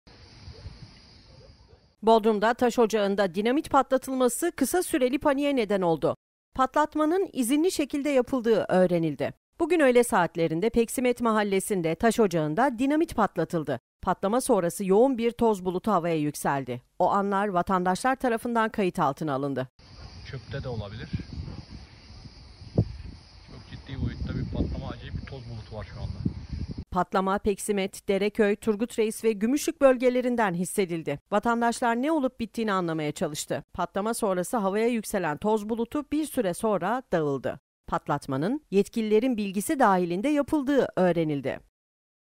KISA SÜRELİ PANİK YAŞANDI
Kısa Süreli Panik Yaşandı

Bodrum'da taş ocağında dinamit patlatılması kısa süreli paniğe neden oldu. Patlatmanın izinli şekilde yapıldığı öğrenildi.

Bugün öğle saatlerinde Peksimet Mahallesi'nde taş ocağında dinamit patlatıldı. Patlama sonrası yoğun bir toz bulutu havaya yükseldi. O anlar, vatandaşlar tarafından kayıt altına alındı.

Patlama, Peksimet, Dereköy, Turgutreis ve Gümüşlük bölgelerinden hissedildi. Vatandaşlar, ne olup bittiğini anlamaya çalıştı. Patlama sonrası havaya yükselen toz bulutu, bir süre sonra dağıldı.

Patlatmanın yetkililerin bilgisi dahilinde yapıldığı öğrenildi.